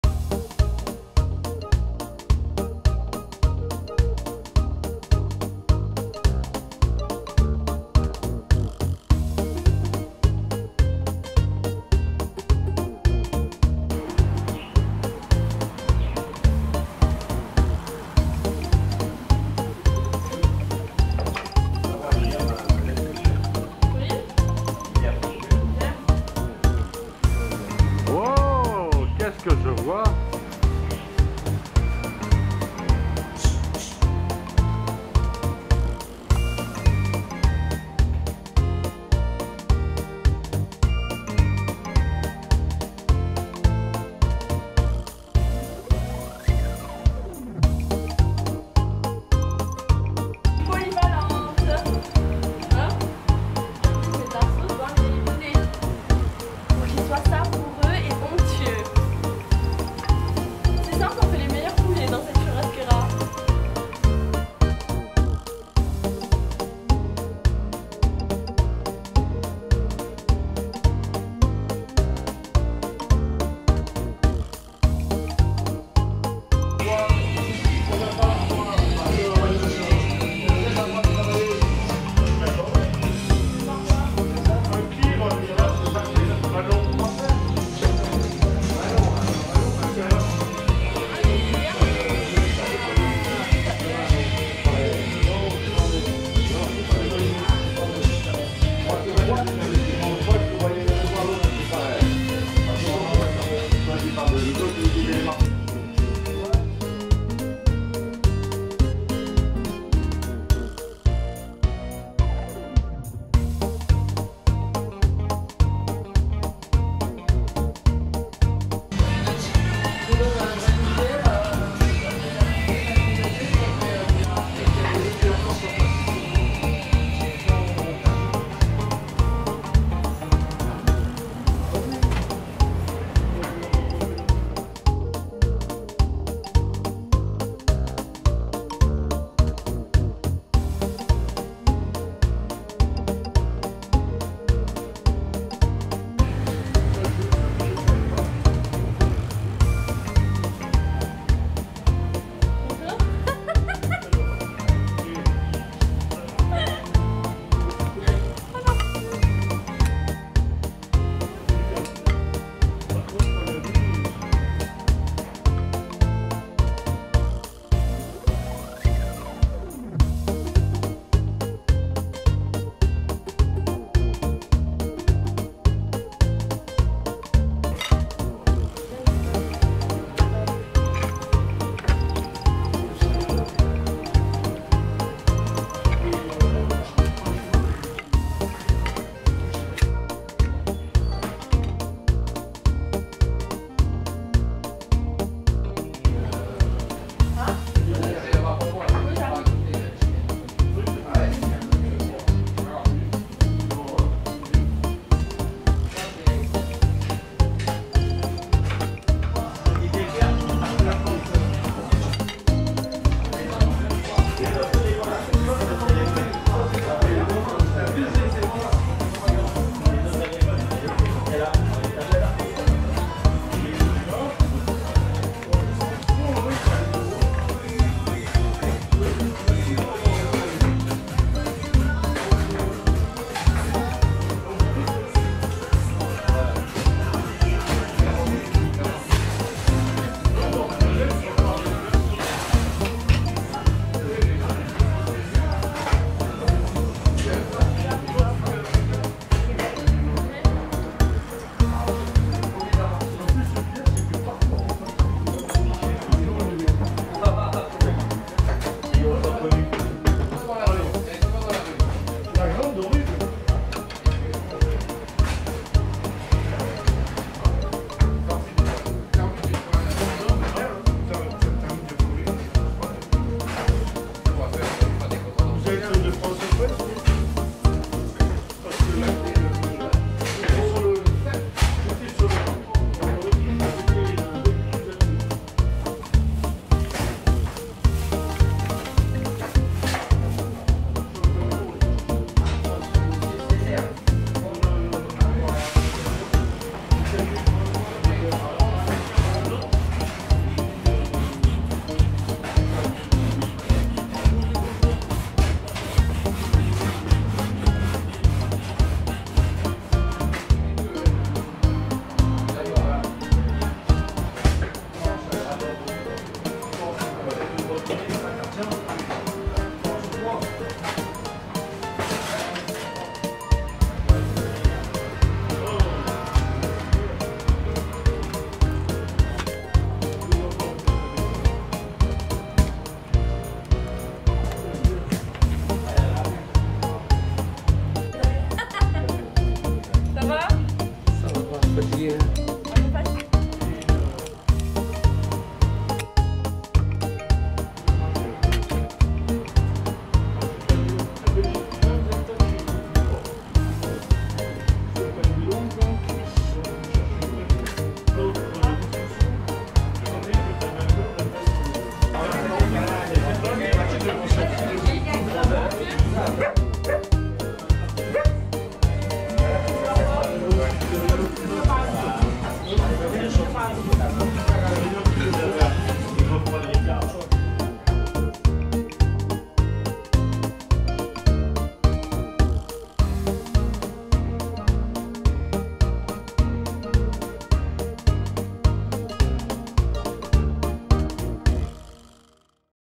Poulet Churrasquiera
La spécialité de l'établissement LE PLAISANCE, est le poulet façon churrasquiera préparé par Mila.
A especialidade do estabelecimento O prazer é o frango maneira churrasquiera preparado por Mila.